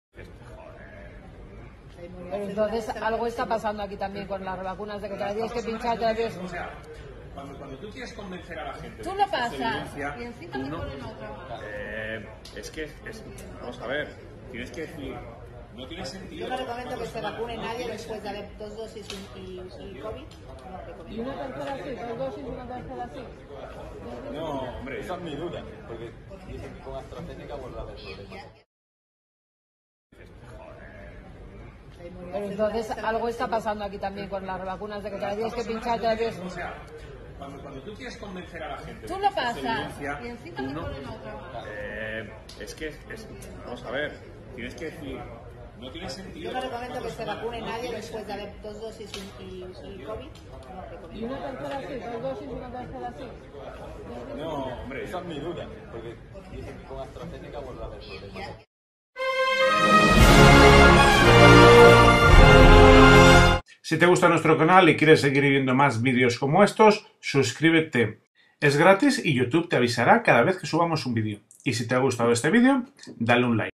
Lo que OPINAN los médicos pro💉 de la 3 Dosis
PILLADÍSIMA
Los médicos pro vac. dicen una cosa en público... pero cuando están en un intermedio... OIDLOS